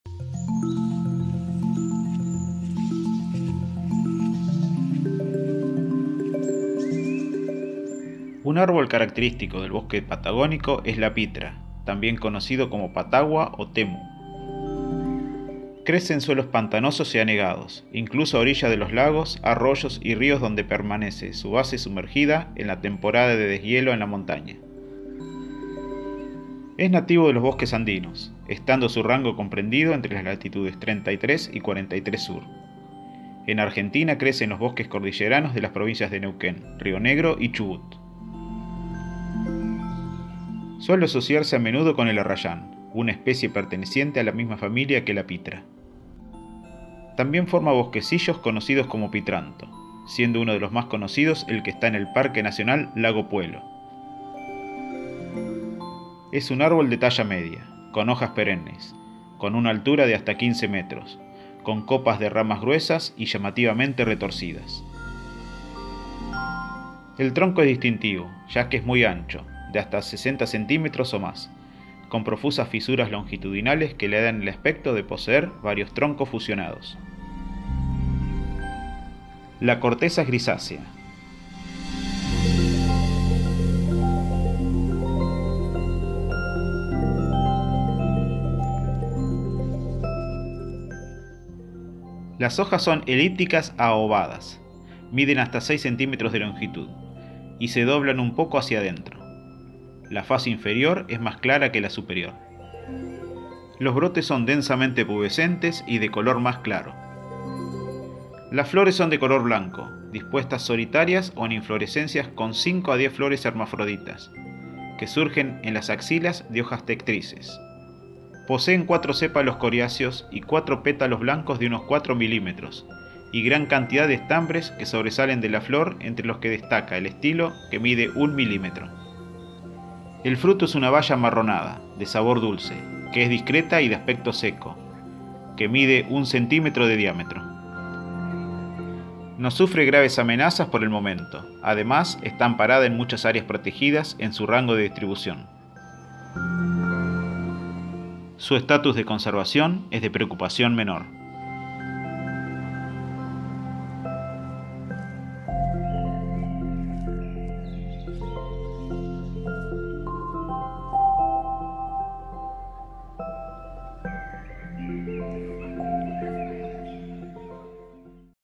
Serie Árboles Nativos de Argentina - Pitra (Myrceugenia exsucca) - Capítulo 14
Decimocuarto capítulo de la serie sobre árboles nativos de Argentina y en esta ocasión es el turno de la pitra, árbol nativo de los Andes del centro de Argentina y Chile, perteneciente a la familia Myrtaceae.
Datos sobre la especie y su estatus de conservación.

Para ayudar al canal colabora con un cafecito (Argentina)

Esto es de gran ayuda para que el canal siga creciendo.